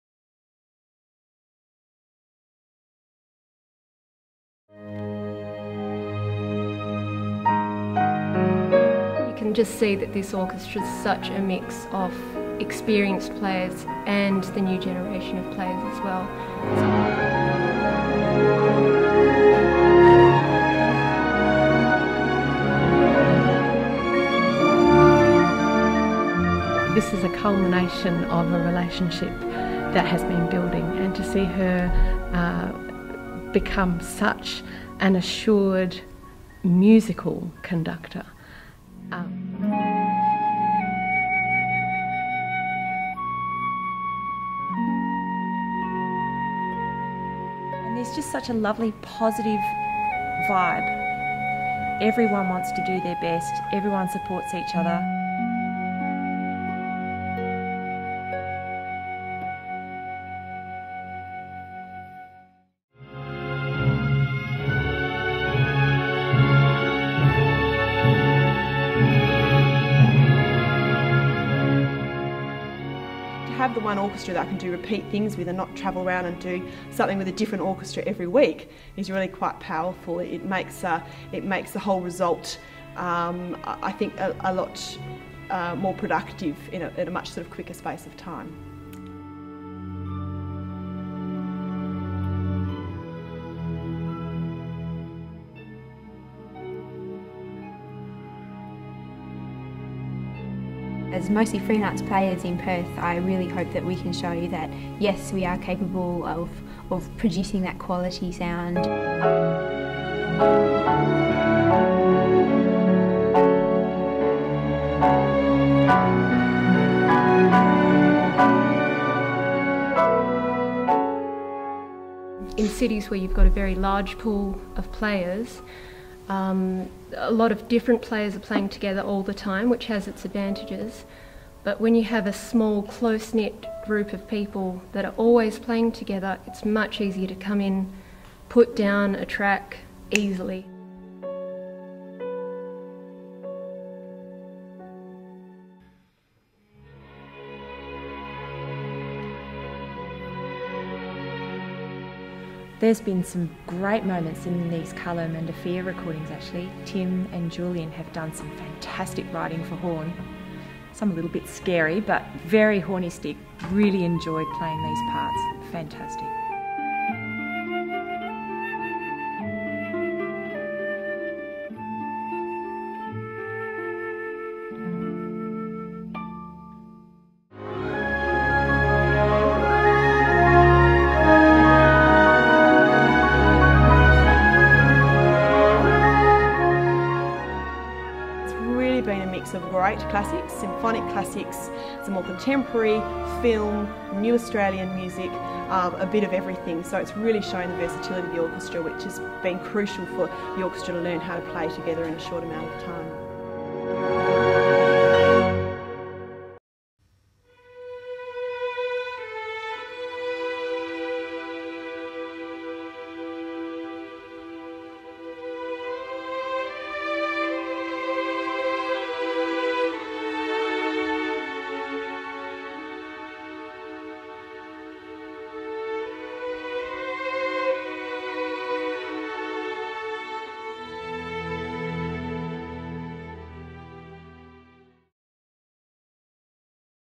PSO behind the scenes ABC Studio
Interviews and snippets from the Perth Symphony Orchestra's latest recording sessions of works by Carlo Mandofia, assisted by Tim Count, orchestrated and arranged by Julian Kershaw and conducted by Jessica Gethin. Thanks to Karl Akers and Guy Smith for engineering. Soloists include Adam Pinto (piano), Michael Howell (flute) and Anthony Maydwell (harp).